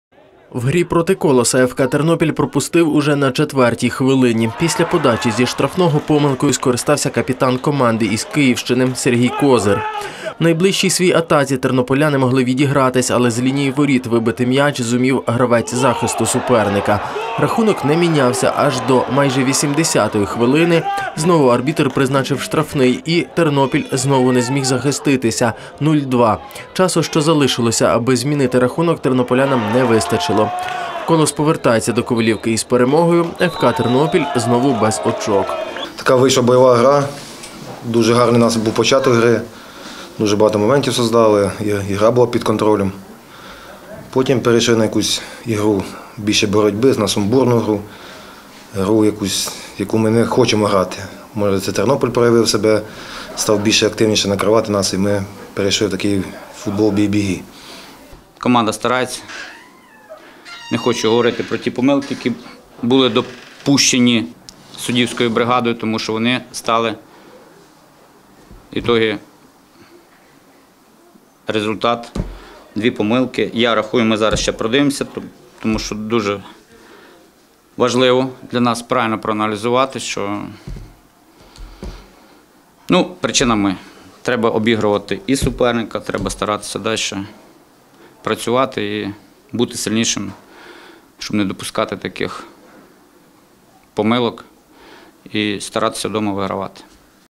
ФК "Тернопіль" поступився "Колосу" із Ковалівки (Відео)
ФК "Тернопіль" досі без перемог у першій лізі. Вдома підопічні Івана Марущака не змогли обіграти "Колос" із Ковалівки. Поразка 0:2. За п'ять турів у чемпіонаті тернополяни мають всього одне очко, та посідають останнє місце у турнірній таблиці.